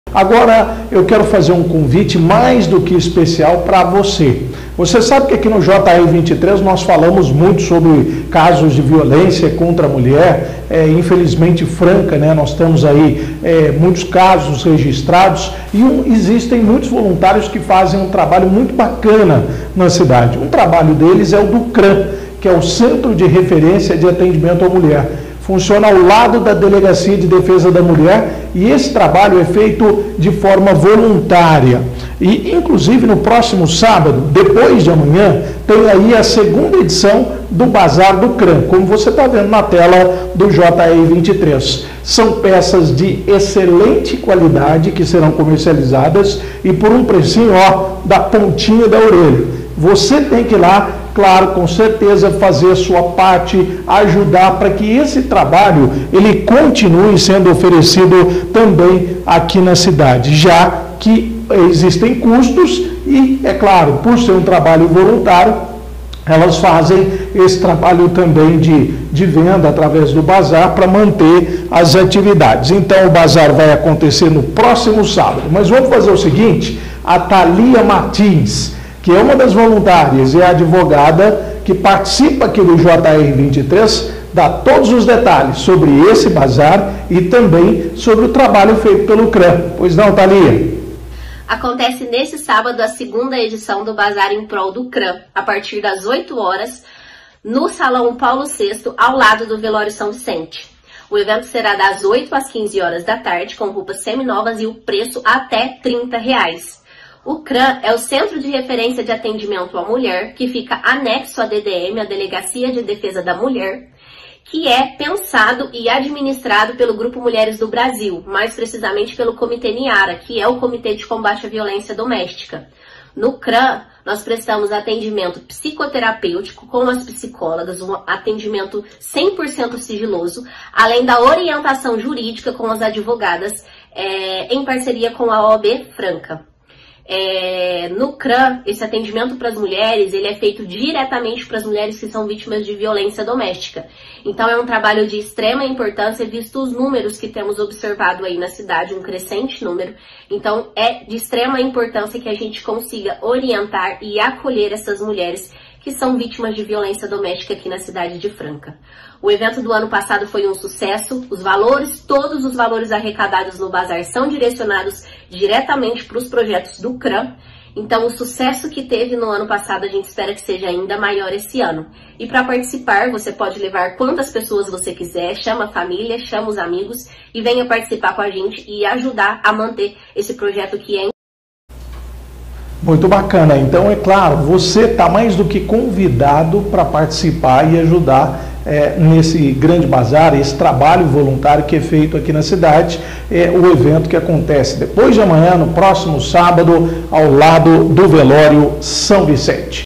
CRAM realiza 2ª Bazar neste sábado em Franca
Thalia Martins, advogada e voluntária CRAM, fala sobre a realização do importante evento, na edição de quinta-feira, 5 de Maio de 2023.